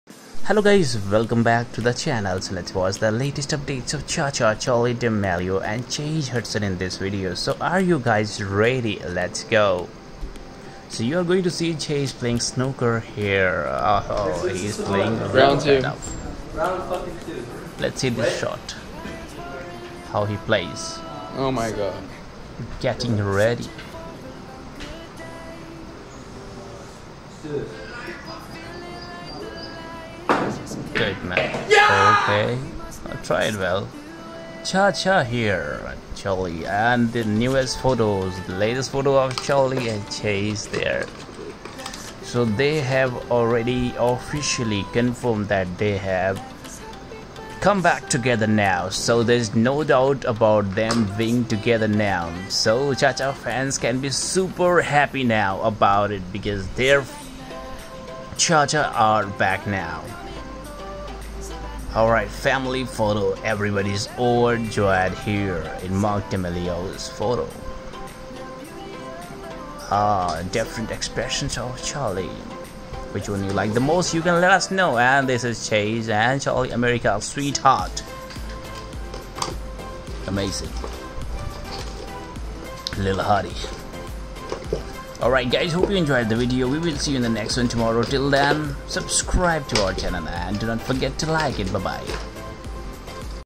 Charli D'amelio & Chase Hudson's New Romantic Vlog Video ! Vlog & News | 21st July , 2021
1. Chase Hudson  Latest News
2. Charli Da'melio Latest News
3. Chase Hudson  and Charli  Da'melio Together at iheart Radio & America's Sweetheart
5. Chase Hudson   New TikTok
6. Charli  Da'melio New TikTok
7. Charli  Da'melio New Instagram Updates / Stories
8. Chase Hudson New Instagram Updates & Stories

Charli  D'amelio and Chase Hudson's Latest Hot  Live Vlog and Instagram Updates | Charli D'amelio's 16th Birthday Celebration |  19th June , 2021